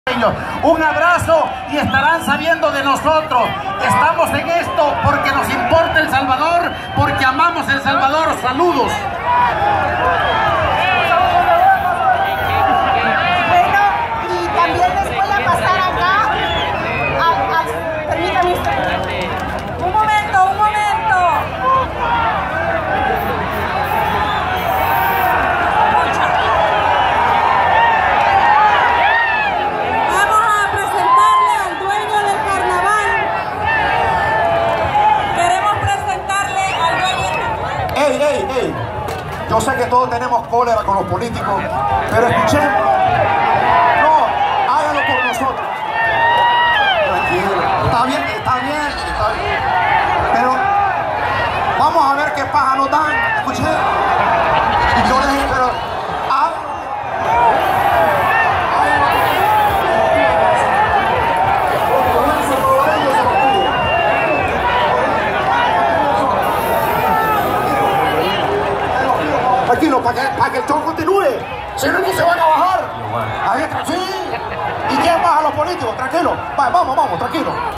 Diáspora salvadoreña abuchea a políticos en el Festival Guanaco en Washington
Josué Alvarado promotor del nuevo partido de VAMOS, Reynaldo Cardoza, Diputado del &nbsp;PCN de Chalatenango, Abilio Menjívar, diputado de GANA por La Unión, Guillermo Portillo, candidato a diputado de ARENA por San Salvador, Hugo Salinas, candidato alcalde por GANA en Intipucá, entre otros, son abucheados en el Festival Guanaco de Washington.